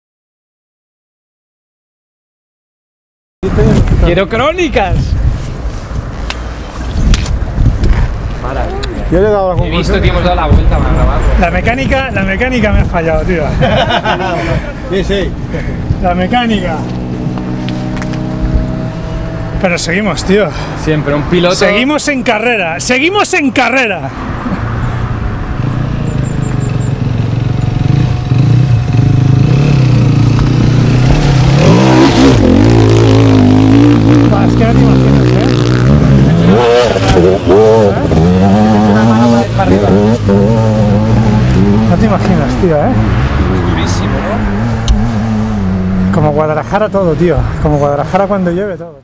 Rafa Torres, el Piloto, sigue en carrera. Nacional de Enduro 2011, Valls de Torruella
os trae en exclusiva las imágenes de El Piloto, Rafa Torres, el cronista del enduro, luchando por seguir adelante en el Nacional de Enduro. La carrera de Valls de Torruella marcó un antes y un después en su trayectoria como líder de masas. Un documente de valor incalculable que queremos compartir con todos nuestros lectores.